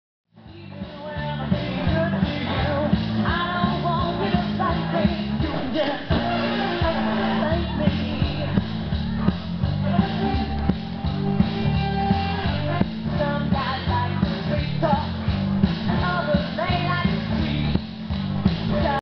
International Dance Night.
International Dance Night at Allegro Puerto Plata, Dominican Republic.
I had other video's of the dances, but this was the only one that I can find. :\ No. I'm not a lesbo or anything like that, it's just ironically the only one that's working. so don't waste your time with stupid comments.